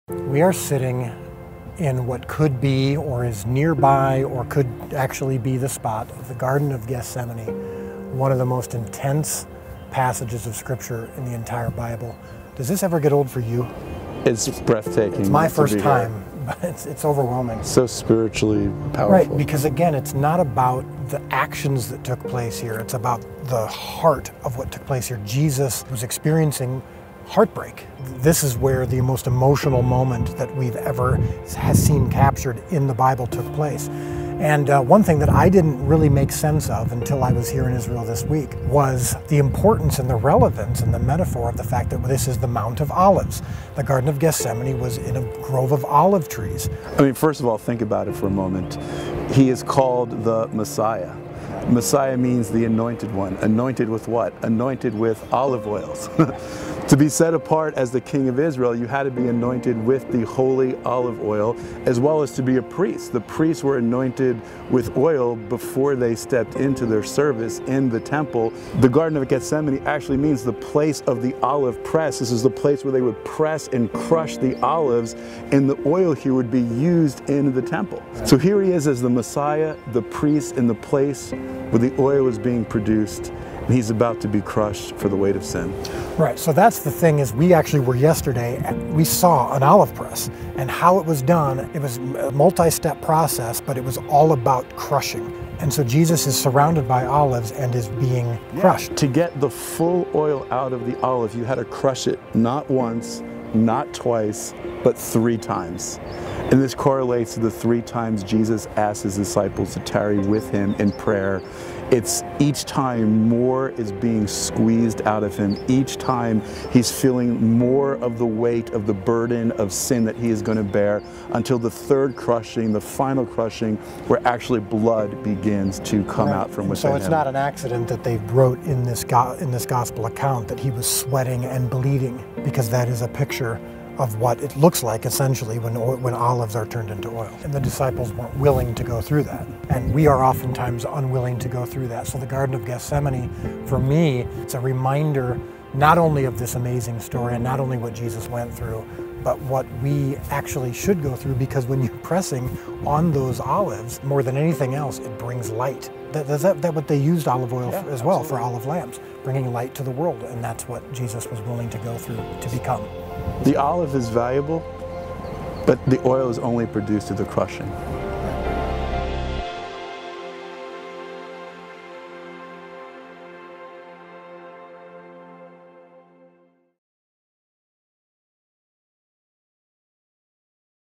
Garden of Gethsemane: the surprising significance of the olive trees
As you celebrate the victory of Easter, it's vital to understand the significance of the Garden of Gethsemane. Before we produced The Chosen, we went to Israel to go deeper into the stories we're telling you.

The meaning and importance of olive trees truly impacted us.